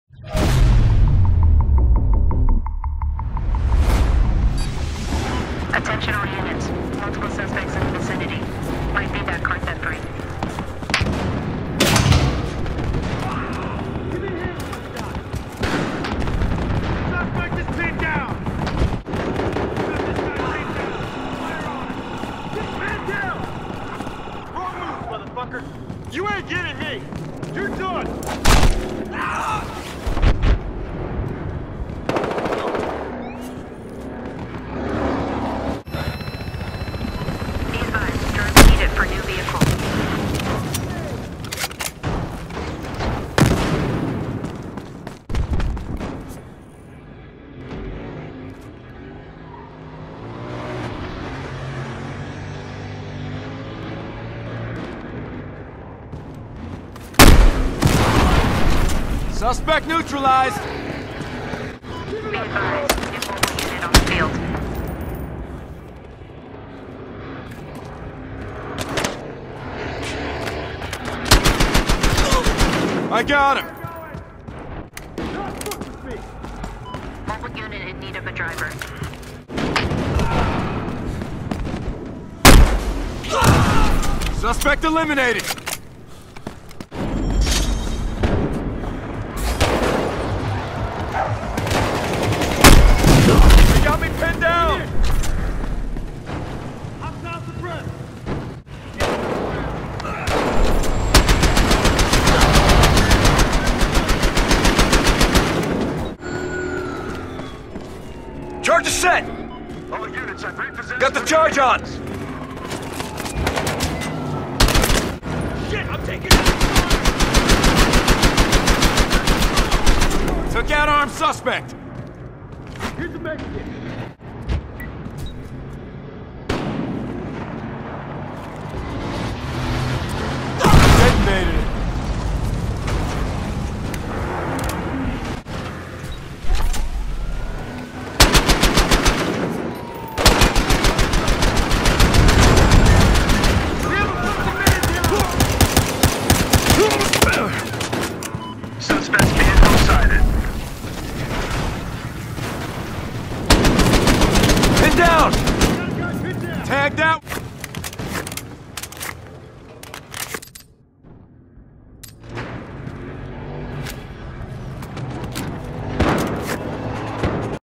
How To Blow Stuff Up
So I had quite a good run in a game and though id make a video about it, tried to make it helpful with tips and pop in a few gags too... hope you enjoy it...